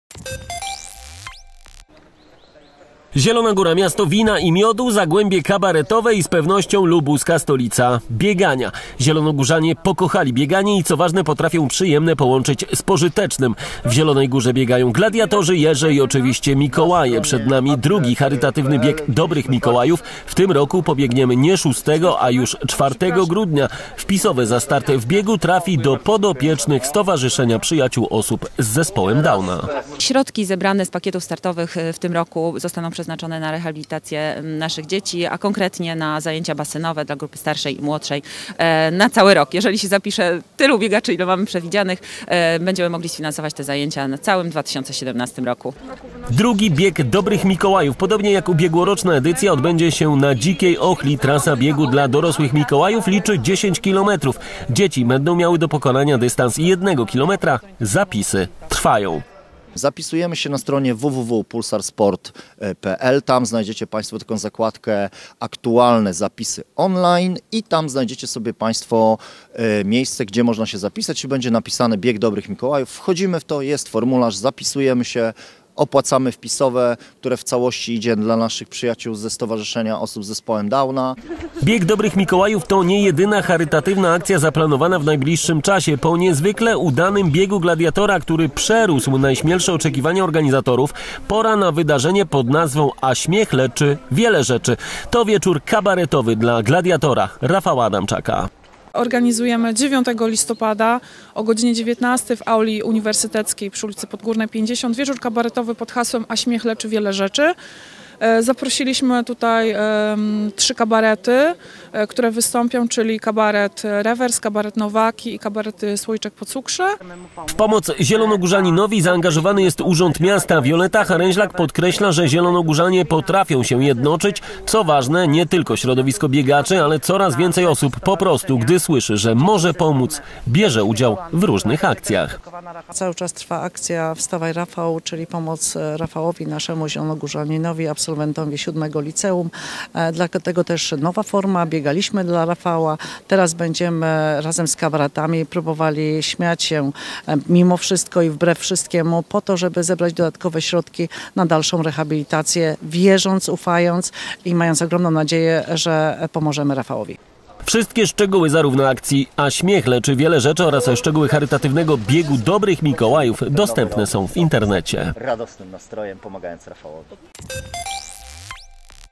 TvZG.pl - Zielona Góra charytatywnie
Zielona Góra miasto wina i miodu, zagłębie kabaretowe i z pewnością, lubuska stolica biegania. Zielonogórzanie pokochali bieganie i co ważne, potrafią połączyć przyjemne z pożytecznym. W Zielonej Górze biegają: gladiatorzy, jeże i oczywiście mikołaje. Przed nami II Charytatywny Bieg Dobrych Mikołajów. W tym roku pobiegniemy nie szóstego, a już czwartego grudnia. Wpisowe za start w biegu trafi do podopiecznych Stowarzyszenia Przyjaciół Osób z Zespołem Downa. Bieg Dobrych Mikołajów podobnie jak w roku ubiegłym odbędzie się na Dzikiej Ochli. Trasa biegu dla dorosłych Mikołajów liczy 10 kilometrów. Dzieci będą miały do pokonania dystans kilometra. Zapisy trwają. Bieg Dobrych Mikołajów, to nie jedyna charytatywna akcja zaplanowana w najbliższym czasie. Po niezwykle udanym Biegu Gladiatora, który przerósł najśmielsze oczekiwania organizatorów, pora na wydarzenie pod nazwą „A śmiech leczy wiele rzeczy” - to będzie wieczór kabaretowy dla gladiatora – Rafała Adamczaka.